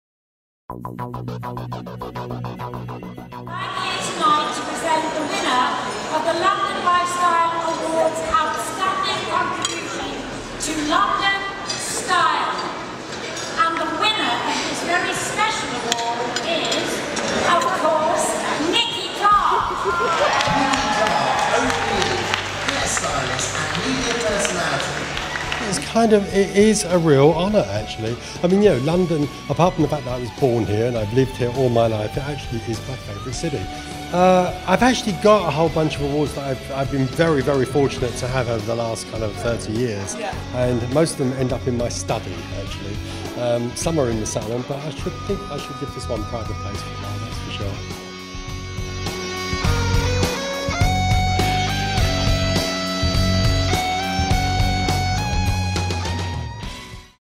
London Lifestyle Awards® 2014 - Outstanding Contribution to London Style to Nicky Clarke
The Award for Outstanding Contribution to London Style 2014 goes to Nicky Clarke.